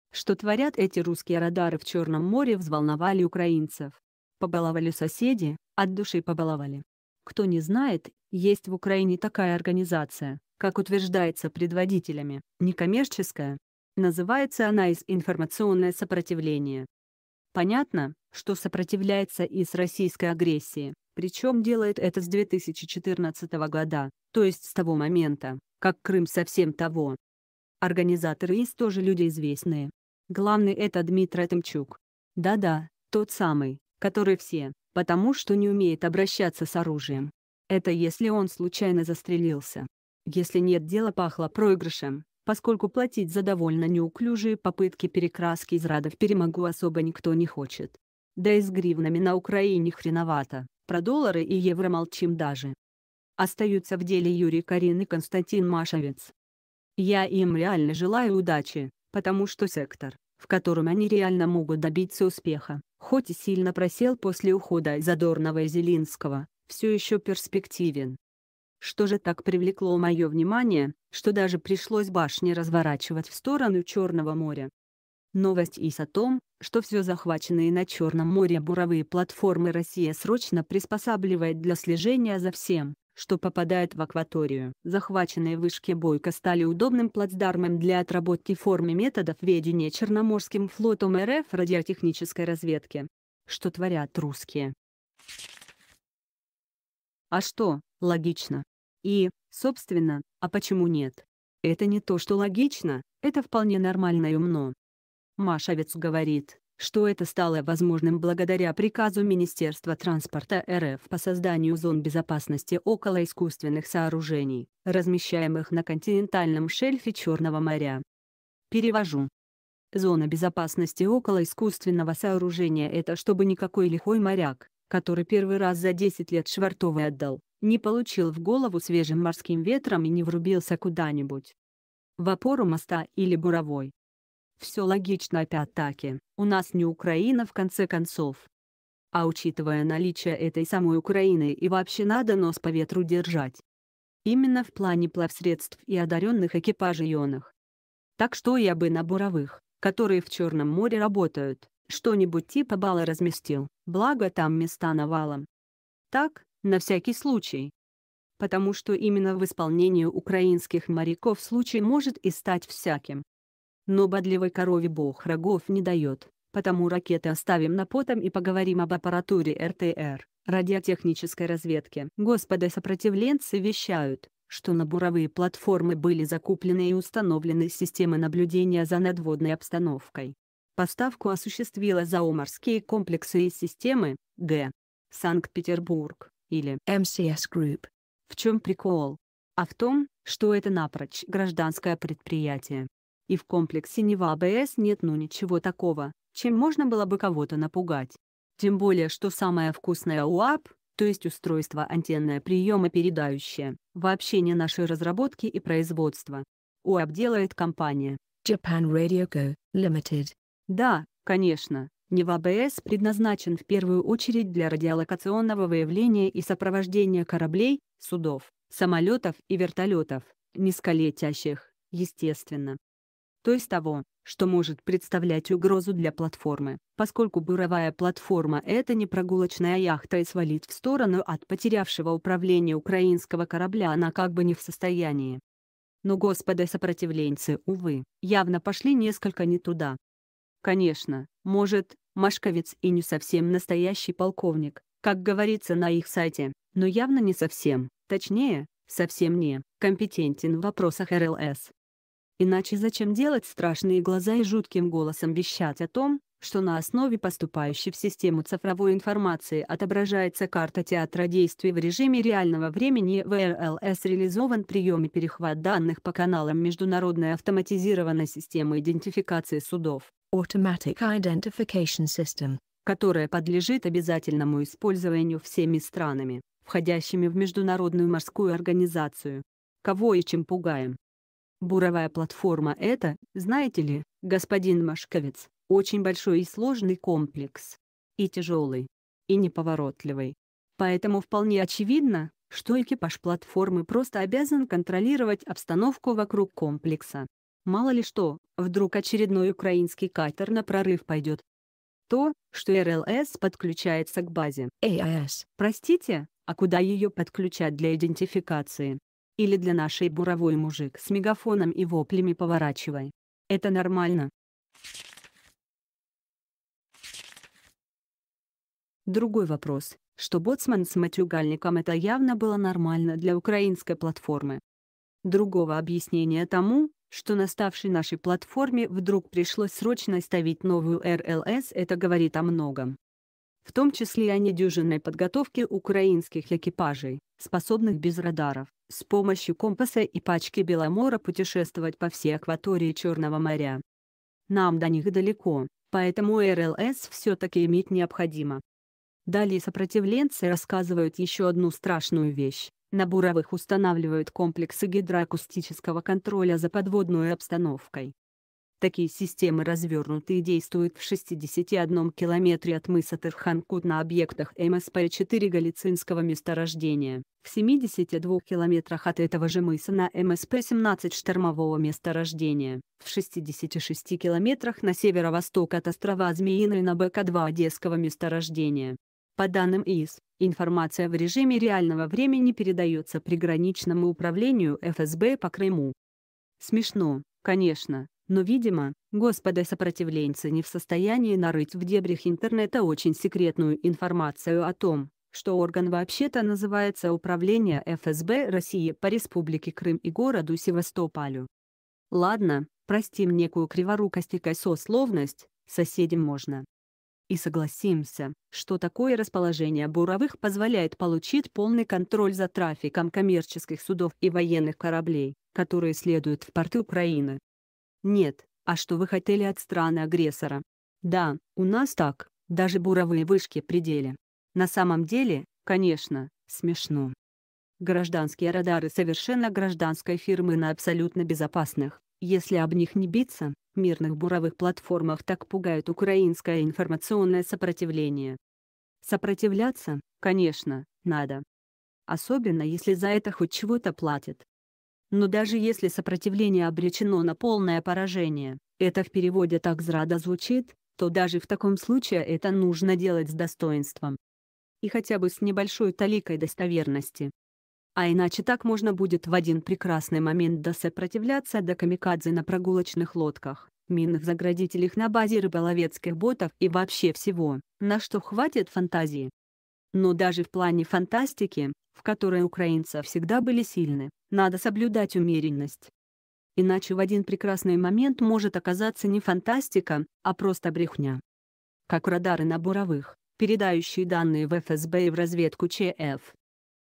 Что творят эти русские радары в Черном море взволновали украинцев
Что творят эти русские радары в Черном море взволновали украинцев Побаловали соседи, от души побаловали . Кто не знает, есть в Украине такая организация, как утверждается предводителями, некоммерческая . Называется она ИС Информационное сопротивление . Понятно, что сопротивляется ИС российской агрессии, причем делает это с 2014 года, то есть с того момента, как Крым совсем того Организаторы ИС тоже люди известные . Главный это Дмитро Тымчук . Да-да, тот самый, который все, потому что не уме